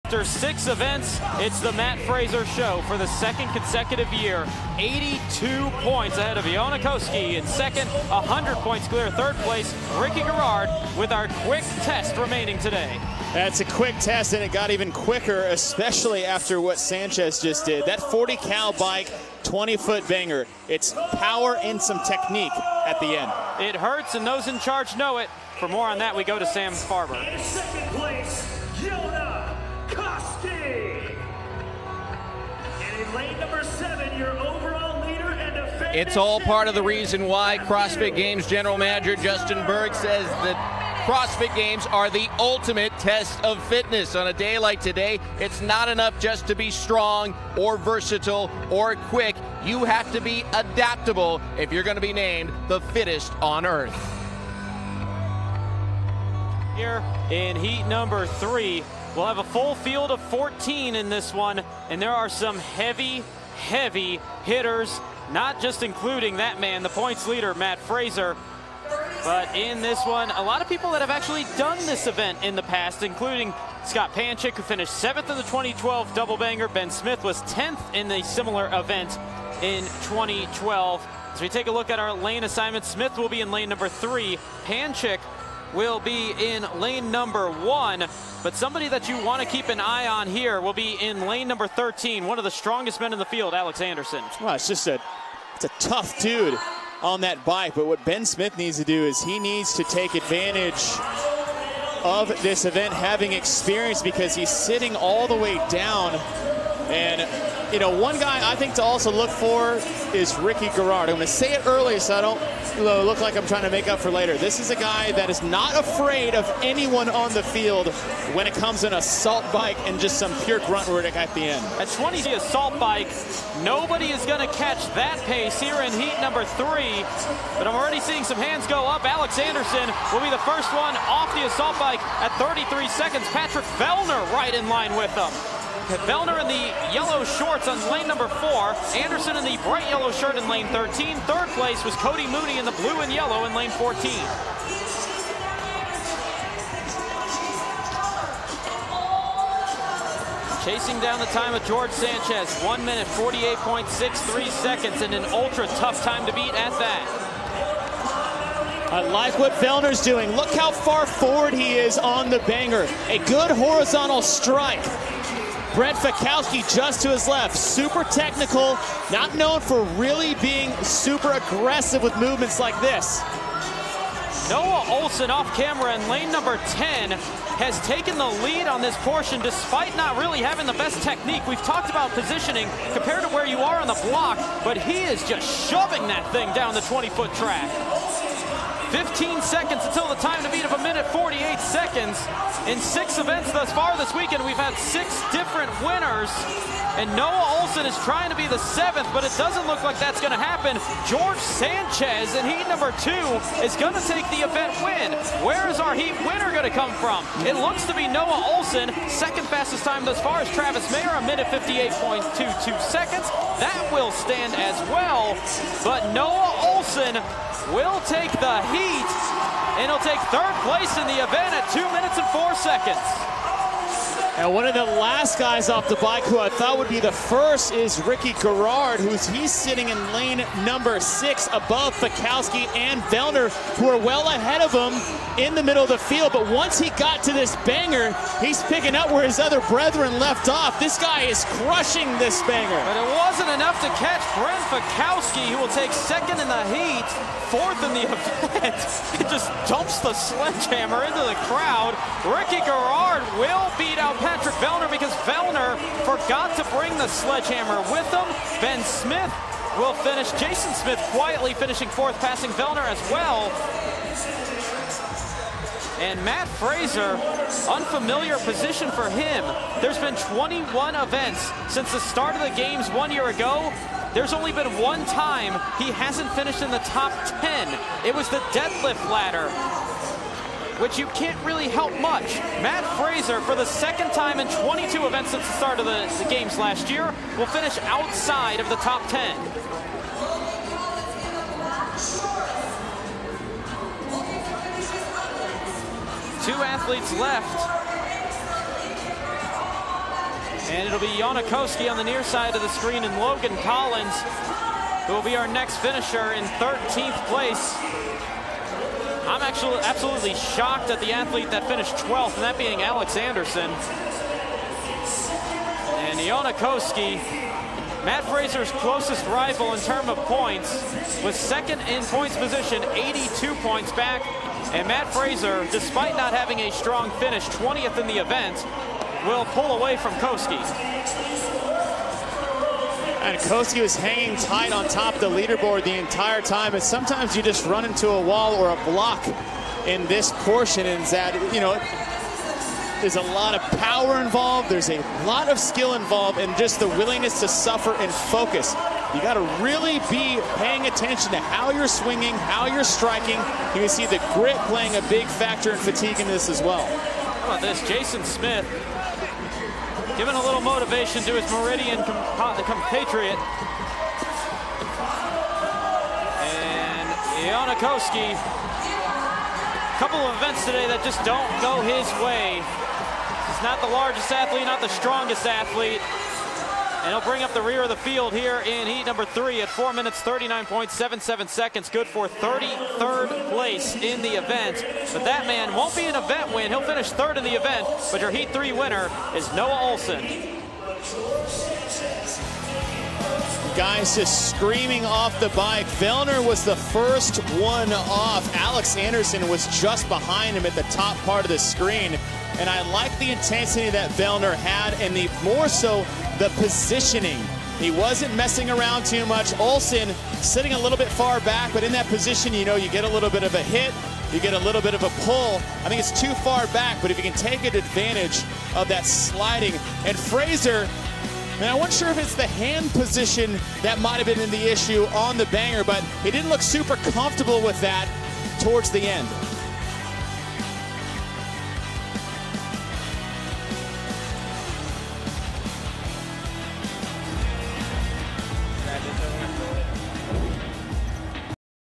Assault Banger Men Event 7 Heart 3 Crossfit Games 2017
For time:
40/30-cal. Assault Bike
20-ft. Banger

Athletes will ride 40 calories (30 for women) on the Assault Bike, then pick up the sledgehammer and hit the Banger across the 20-ft. track, then move to the finish line.

Time cap: 6 minutes